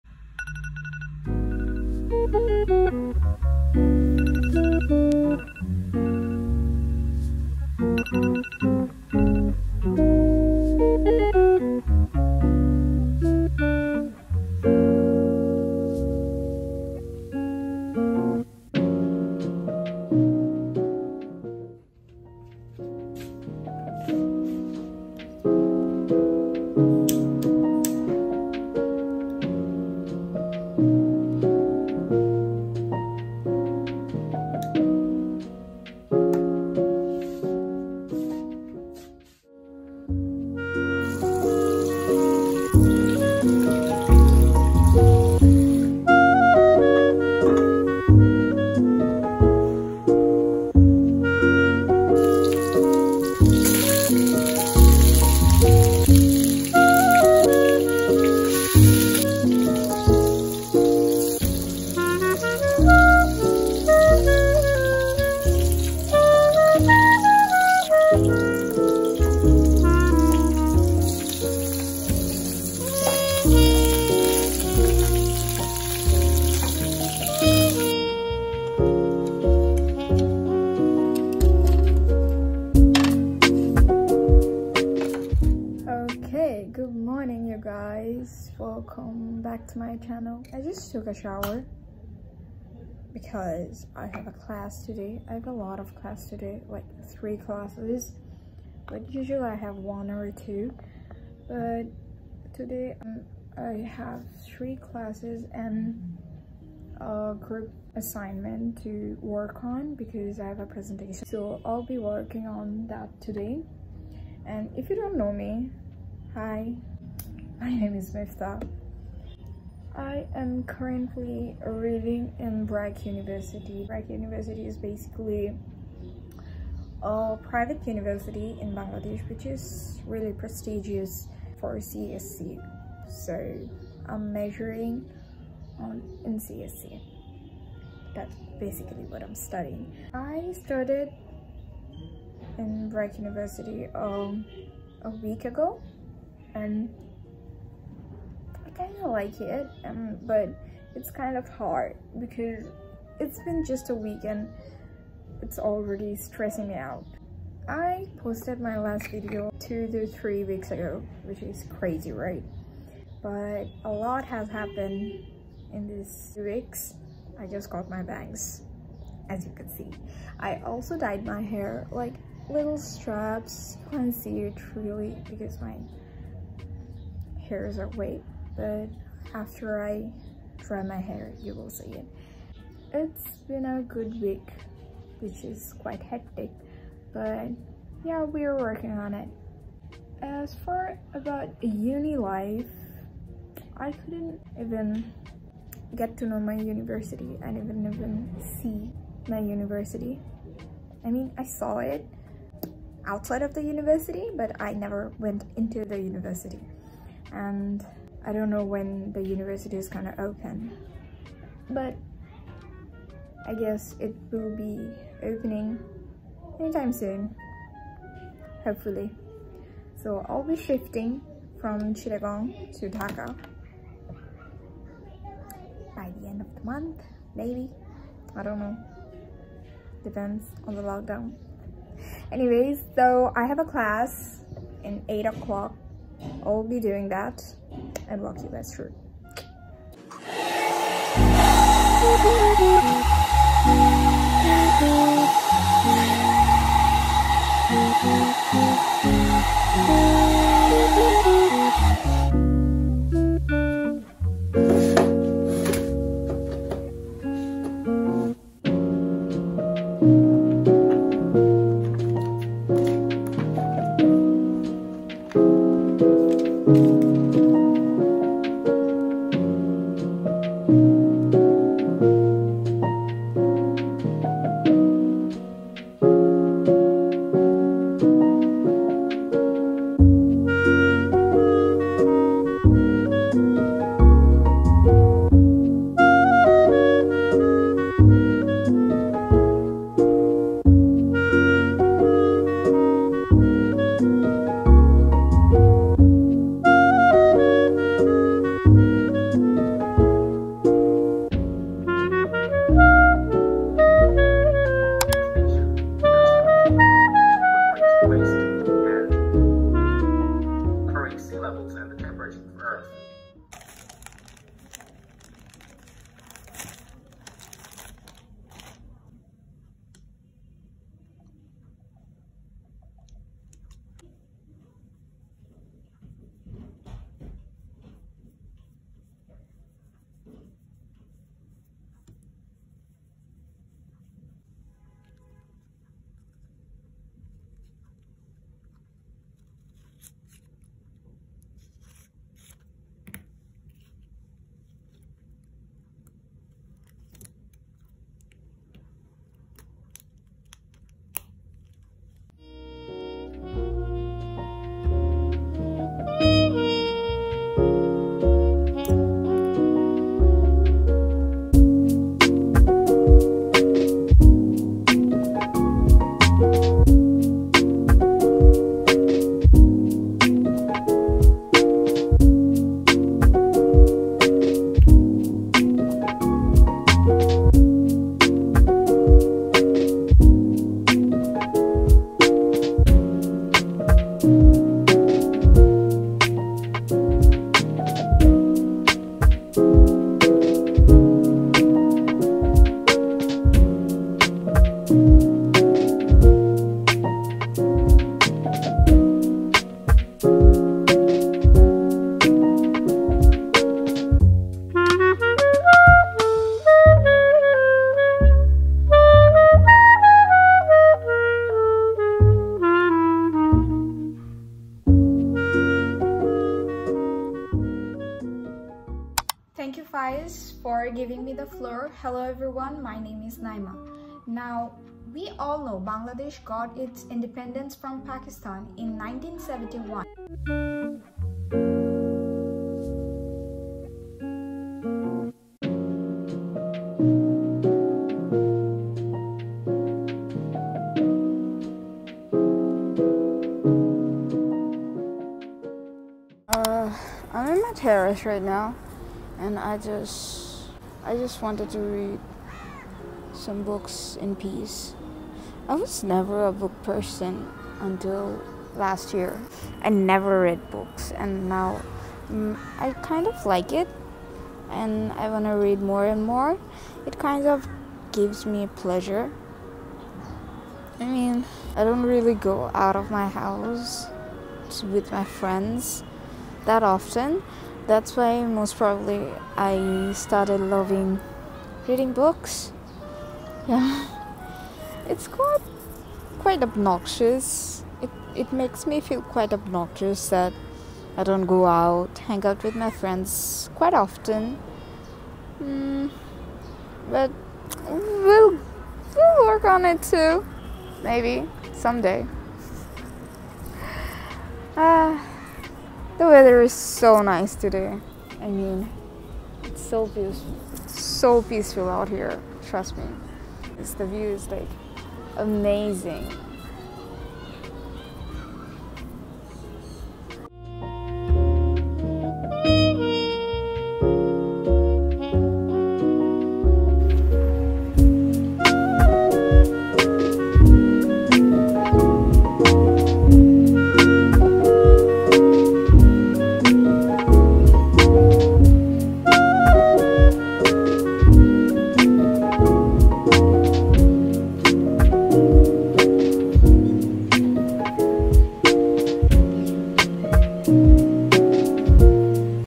Uni life: 📚A day in my life as Brac university fresher “online edition”
This was a day in my life as fresher of brac university.
 The first week was so much hectic for me to cope up with everything that why couldn’t film at all.
But I hope you guys enjoyed this video